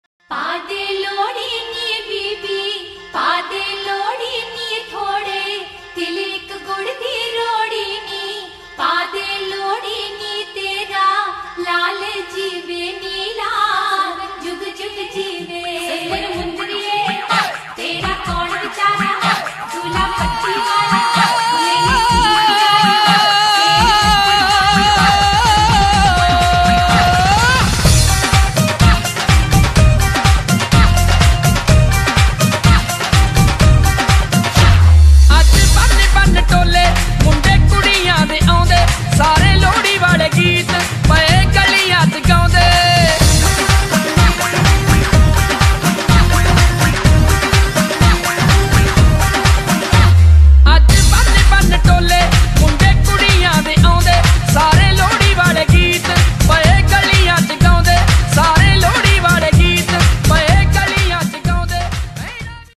Lohri Celebrations at BPR Sr Sec School Pundri kaithal Haryana India
with Lohri Sankalp to help others in need